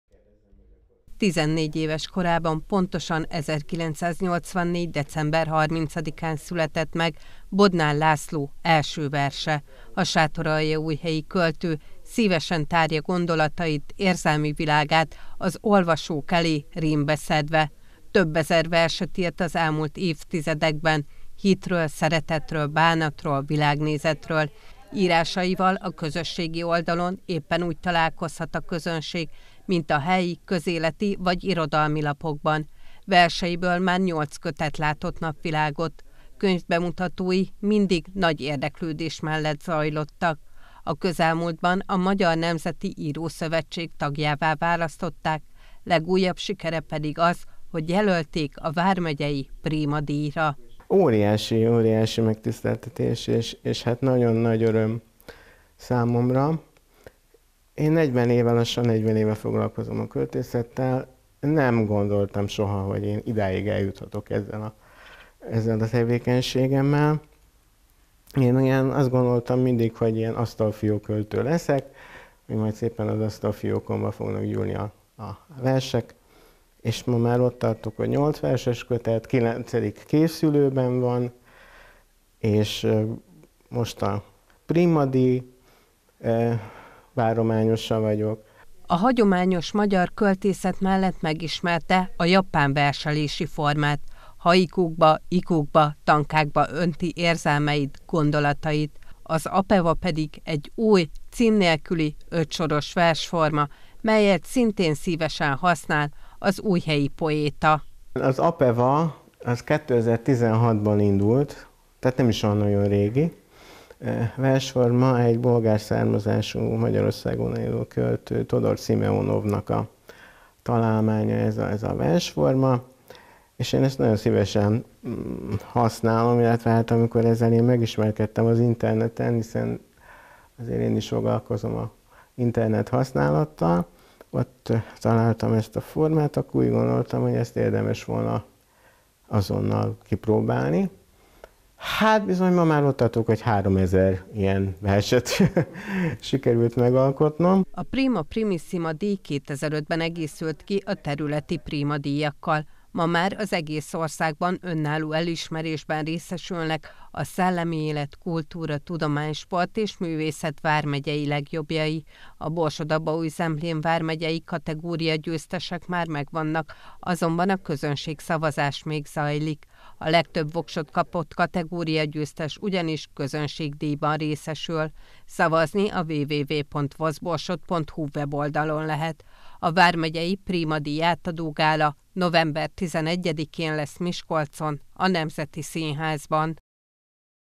Bodnár László sátoraljaújhelyi költőt is jelölték a Vármegyei Prima díjra
Sátoraljaújhelyi költő is van a Borsod-Abaúj-Zemplén Vármegyei Príma-díj várományosok között. Bodnár Lászlóra a magyar irodalom kategória győzteseként lehet szavazni, a közönségdíj elnyerése érdekében. Az újhelyi poéta költeményei helyi, közéleti és irodalmi lapokban jelentek meg. Az elmúlt évben adta ki nyolcadik verseskötetét, de már dolgozik a következő megjelentetésén.